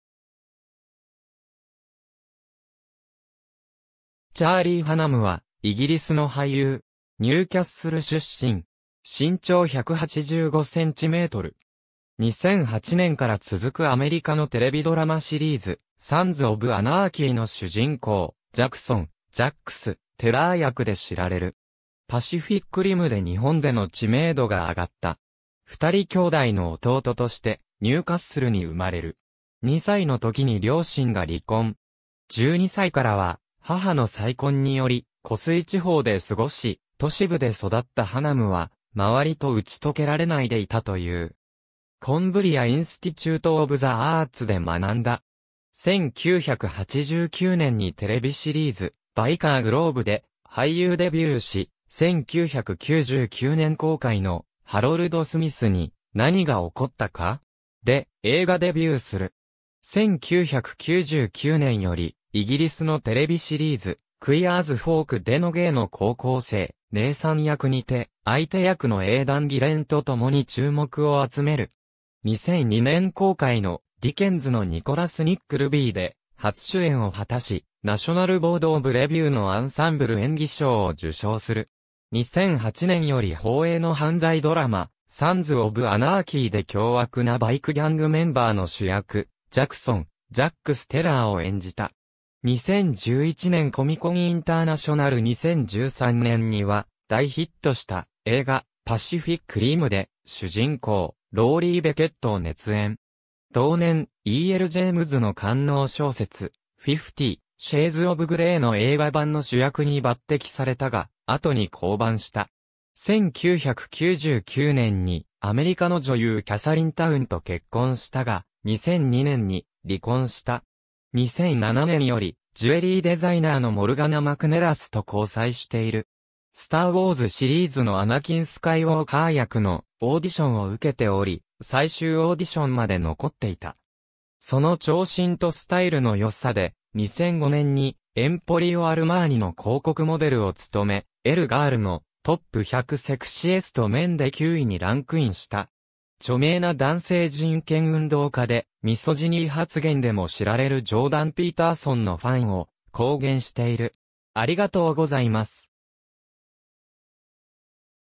チャーリー・ハナム
ニューカッスル・アポン・タイン出身の人物
チャーリー・ハナム（Charlie Hunnam, 本名: Charles Matthew Hunnam, 1980年4月10日 - ）は、イギリスの俳優。
ニューキャッスル出身。
身長185cm。
2008年から続くアメリカのテレビドラマシリーズ『サンズ・オブ・アナーキー』の主人公ジャクソン・“ジャックス”・テラー役で知られる。
『パシフィック・リム』で日本での知名度が上がった。
2人兄弟の弟としてニューカッスルに生まれる。
2歳の時に両親が離婚。
12歳からは母の再婚により湖水地方で過ごし、都市部で育ったハナムは周りと打ち解けられないでいたという。
Cumbria Institute of the Artsで学んだ。
1989年にテレビシリーズ『Byker Grove』で俳優デビューし、1999年公開の『ハロルド・スミスに何が起こったか?』で映画デビューする。
1999年よりイギリスのテレビシリーズ『Queer as Folk』でのゲイの高校生ネイサン役にて、相手役のエイダン・ギレンと共に注目を集める。
2002年公開の『ディケンズのニコラス・ニックルビー』で初主演を果たし、ナショナル・ボード・オブ・レビューのアンサンブル演技賞を受賞する。
2008年より放映の犯罪ドラマ『サンズ・オブ・アナーキー』で凶悪なバイクギャングメンバーの主役ジャクソン・“ジャックス”・テラーを演じた。
2011年 コミコンインターナショナル 2013年には大ヒットした映画『パシフィック・リム』で主人公ローリー・ベケットを熱演。
同年、E.L.ジェームズの官能小説『フィフティ・シェイズ・オブ・グレイ』の映画版の主役に抜擢されたが、後に降板した。
1999年にアメリカの女優キャサリン・タウンと結婚したが、2002年に離婚した。
2007年よりジュエリーデザイナーのMorgana McNelisと交際している。
『スター・ウォーズ』シリーズのアナキン・スカイウォーカー役のオーディションを受けており、最終オーディションまで残っていた。
その長身とスタイルの良さで、2005年にエンポリオ・アルマーニの広告モデルを務め、Elle Girl の「top 100 sexiest men」で9位にランクインした。
著名な男性人権運動家(MRM)でミソジニー発言でも知られるジョーダン・ピーターソンのファンを公言している。